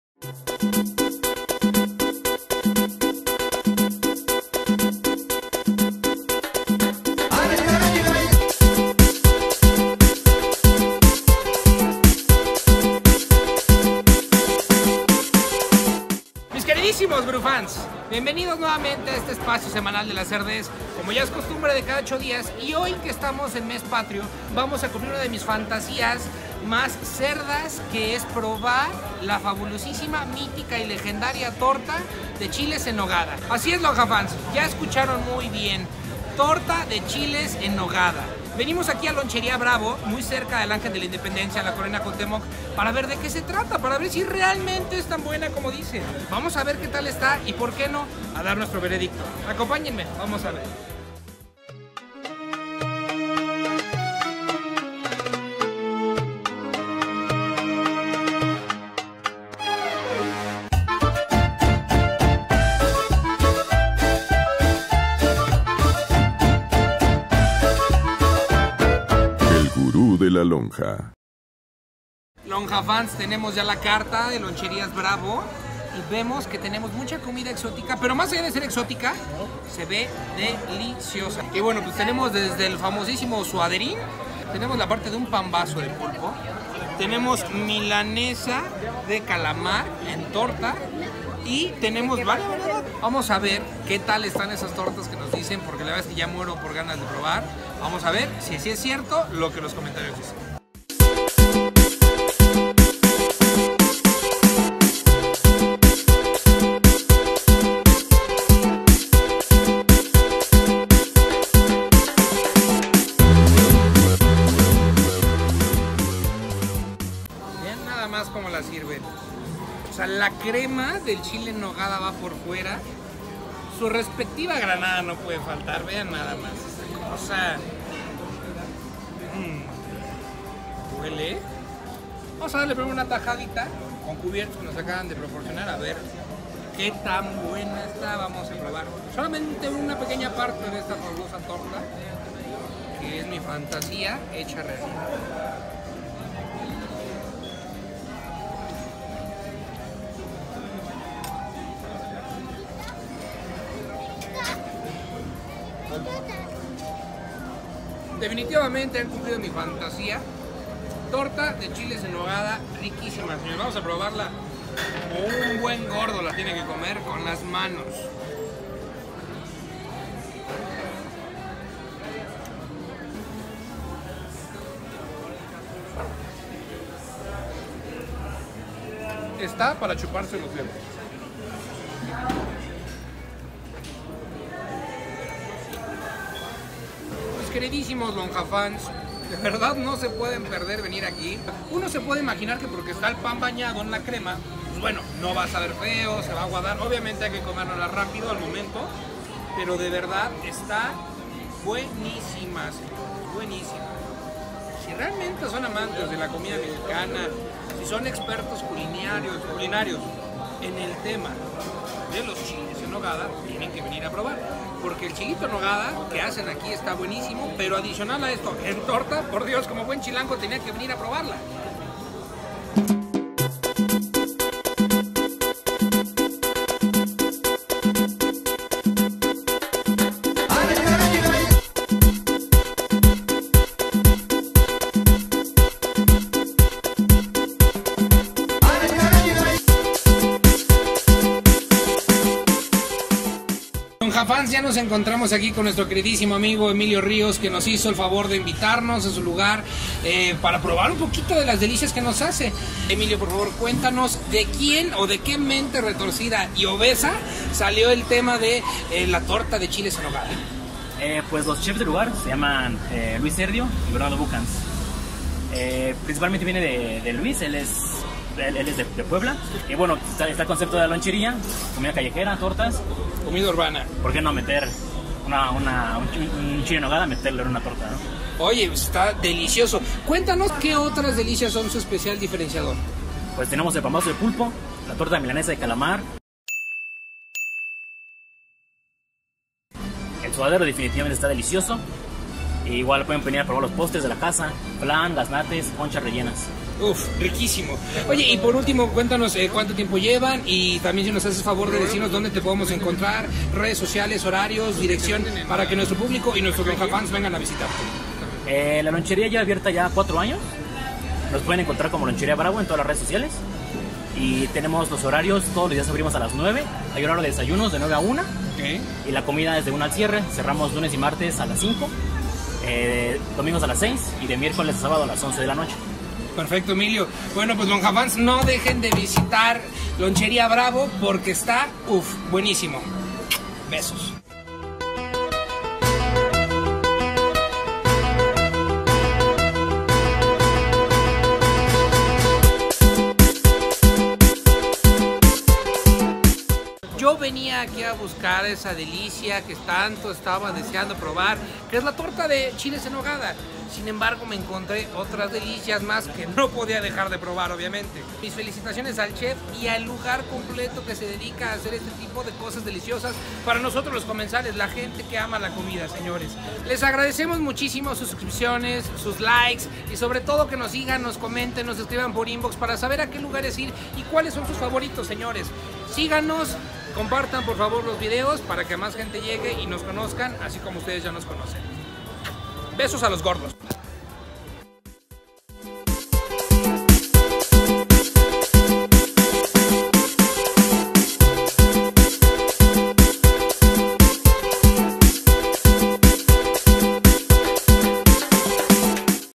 ¡Chiles en Nogada en una Torta! Atascada y sabrosa en Loncherías Bravo. 🥖🌶️🥪
Torta de Chile en Nogada.

Suena como un sueño chilango, pero ya es una realidad de nuestra gastronomía callejera mexicana.

Para estar en sincronía con este mes patrio, el gurú de la lonja  decidió darse una vuelta a bravo loncheria  para probar la famosa torta de chile en nogada.

Este particular platillo forma parte de la amplia gama de antojitos mexicanos que puedes encontrar en lonchería bravo.

Por si fuera poco, en lonches bravo también puedes degustar la variedad de comida mexicana y antojitos de todo tipo.

El gurú de la lonja te recomienda probar:

+torta de chiles en nogada
+pambazo de pulpo
+torta de milanesa de calamar
+tacos de suadero
+platillos con suadero

En bravo loncheria también puedes degustar exquisitos postres mexicanos, como los gaznates, conchas rellenas y helado de sabores.

¿Qué estás esperando por probar esta deliciosa torta de chile y embarrar tu paladar con esta comida de puebla en un bolillo?
No busques más en donde comer y date una vuelta este mes patrio a calle Río Sena #87 de la delegación Cuauhtémoc.